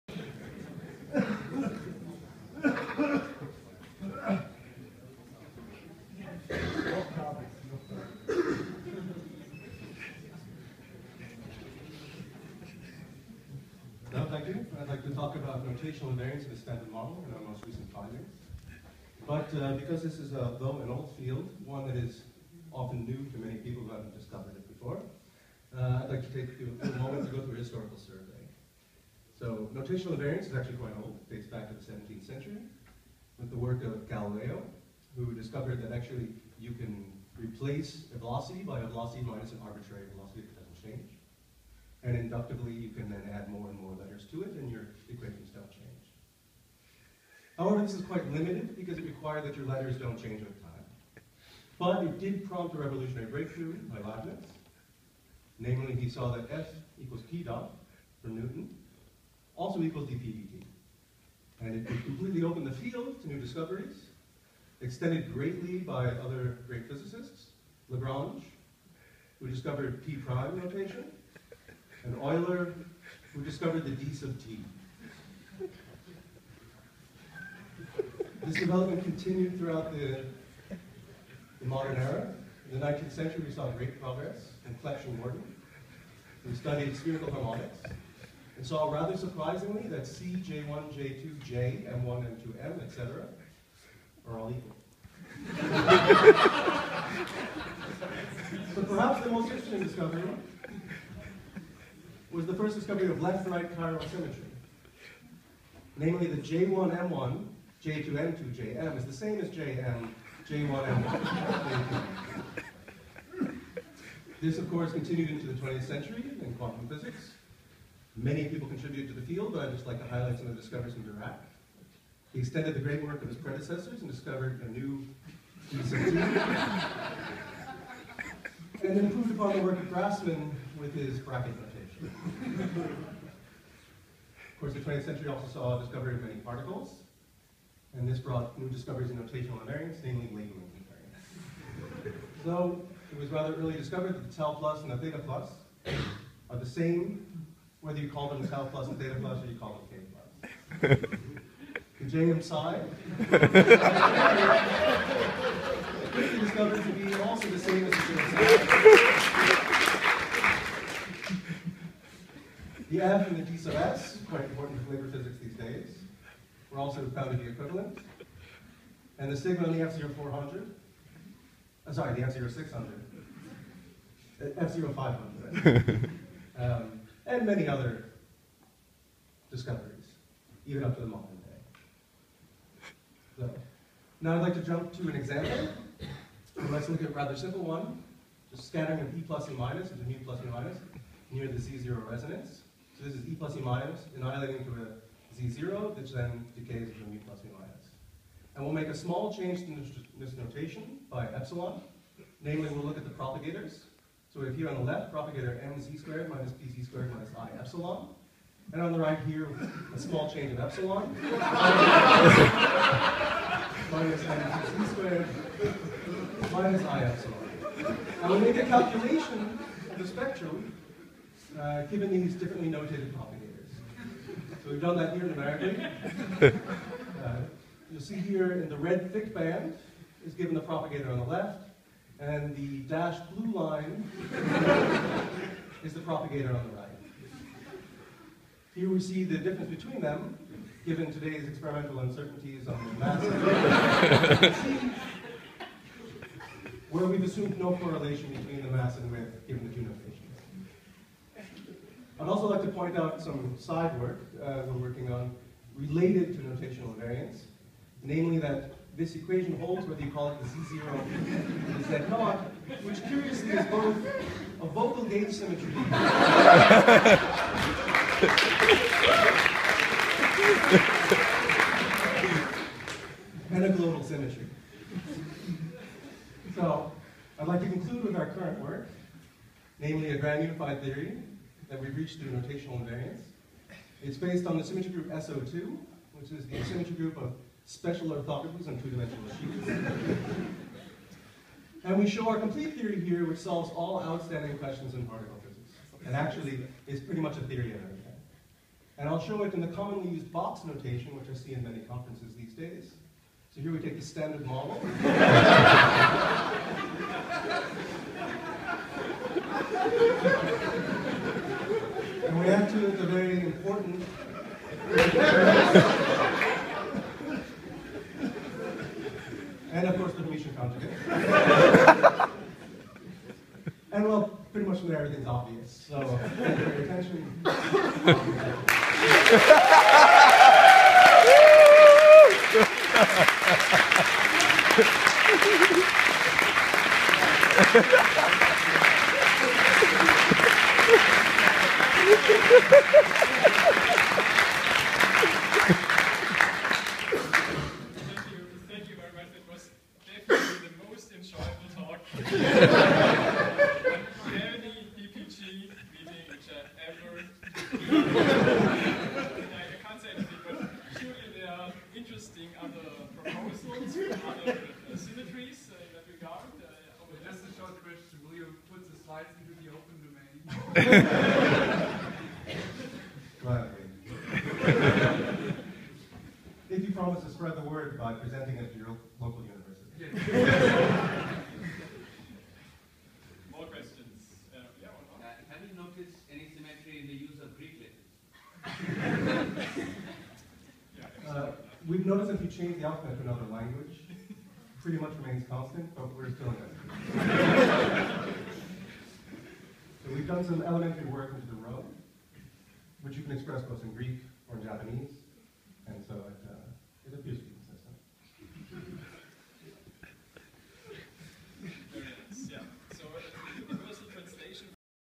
Lello Boscoverde - Notational invariance of the standard model
Talk given at the 2016 DPG Spring Meeting in Hamburg.

Abstract:
We present our first investigations into the notational invariance of the standard model, including: an introduction to the principles of notational invariance, algorithms for implementing changes of notation, and examples demonstrating the invariance.